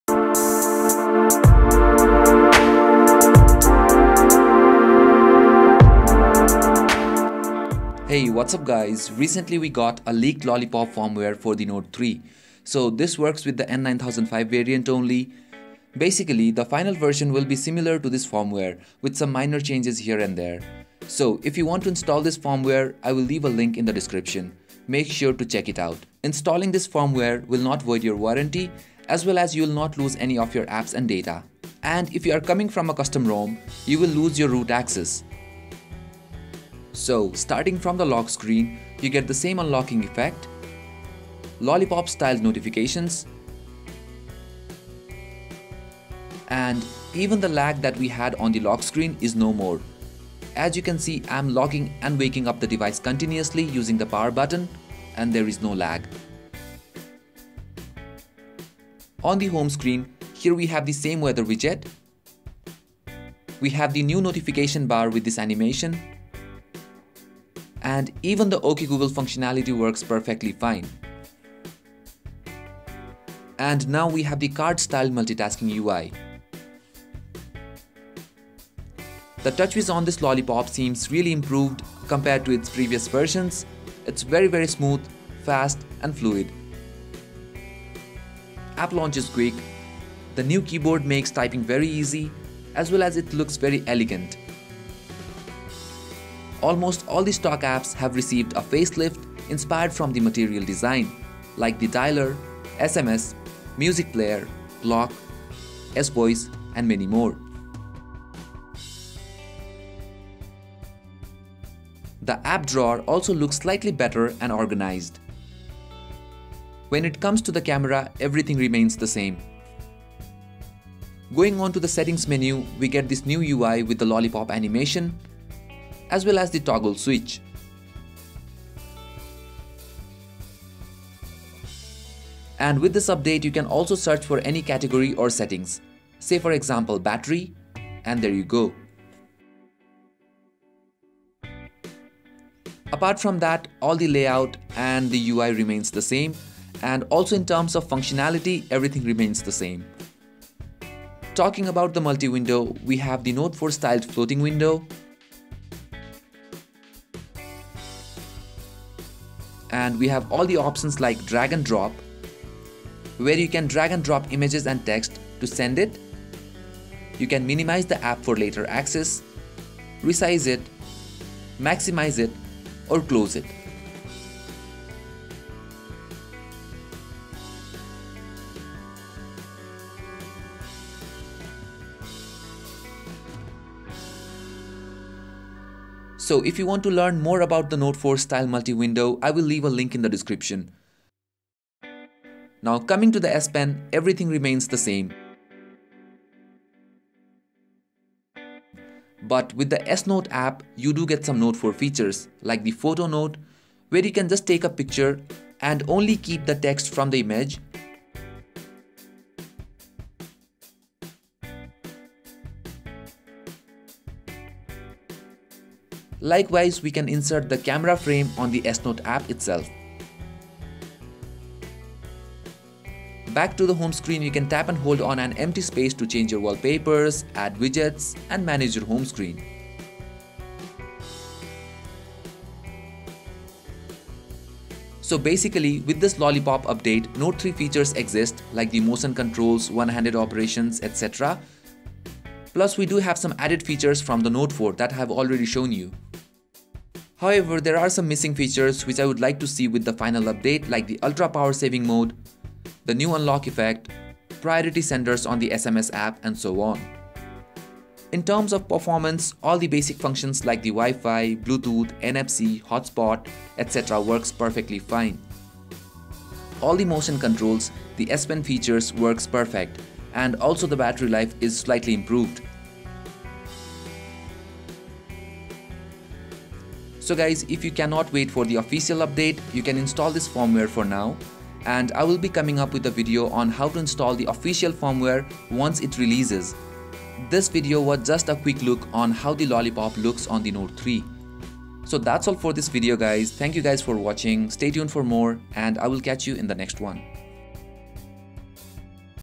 Note 3 - What's new with Android 5.0 Lollipop ? (Leaked)
Here is what's changed with the Lollipop Update on the Note 3. The final update might have some changes. Since this is the leaked firmware.